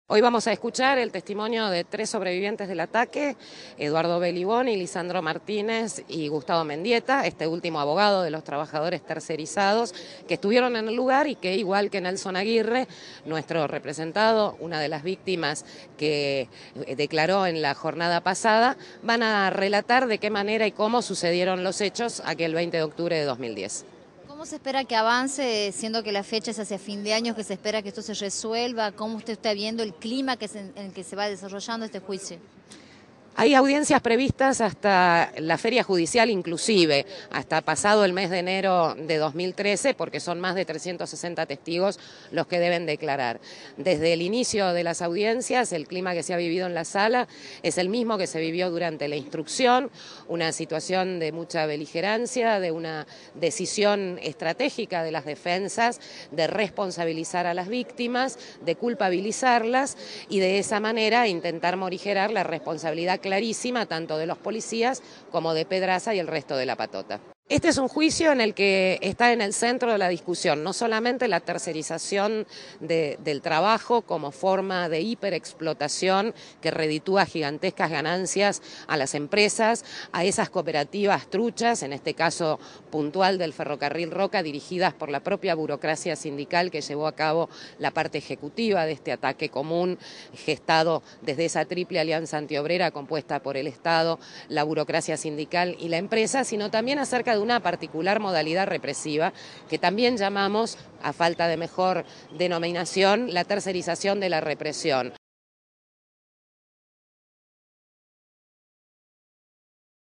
Continúan los testimonios por el crimen de Mariano Ferreyra
María del Carmen Verdú, abogada querellante, dijo hoy  que  "las defensas quieren culpabilizar a las víctimas y morigerar la responsabilidad de Pedraza y de los policías" en el auditorio principal del edificio de tribunales federales de Comodoro Py 2002, en el barrio de Retiro, CABA.

   En la audiencia de hoy ante el Tribunal Oral Criminal 21, está previsto que declaren tres testigos presenciales del asesinato del joven militante  Mariano Ferreyra por una patota ferroviaria, el 20 de octubre de 2010.

    Ellos son Eduardo Beliboni y Francisco Lugo, que compartían militancia en el Partido Obrero con Ferreyra, y el abogado de los ferroviarios tercerizados, Gustavo Mendieta.